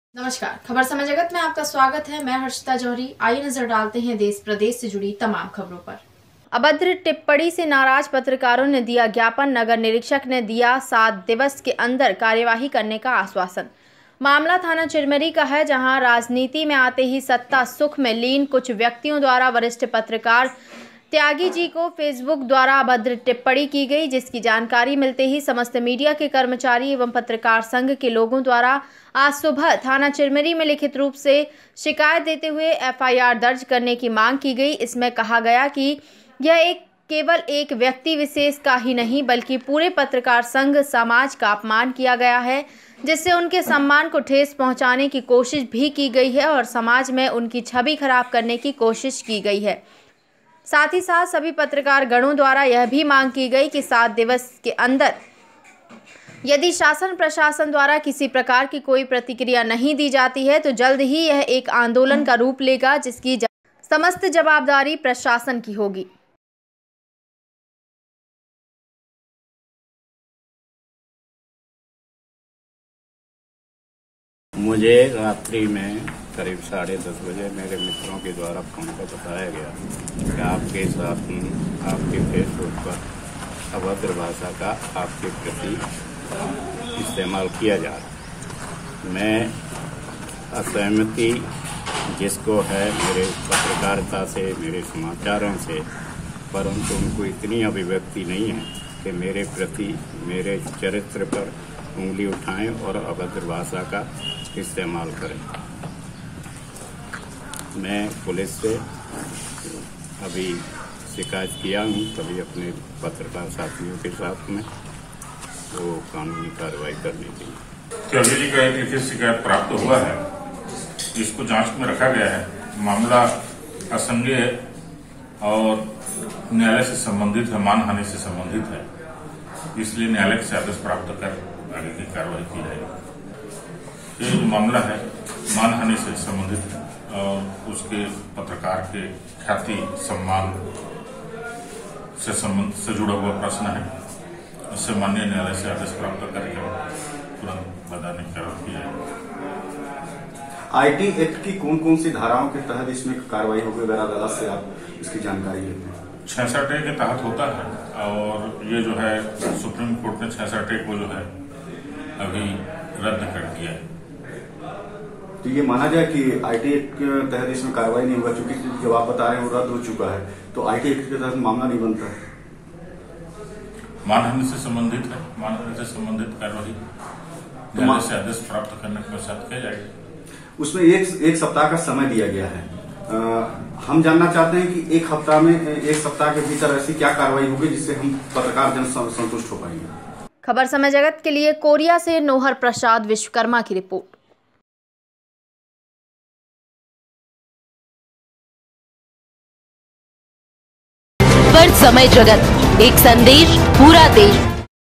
अभद्र टिप्पणी से नाराज पत्रकारों ने दिया ज्ञापन
कोरिया से नोहर प्रसाद विश्वकर्मा की रिपोर्ट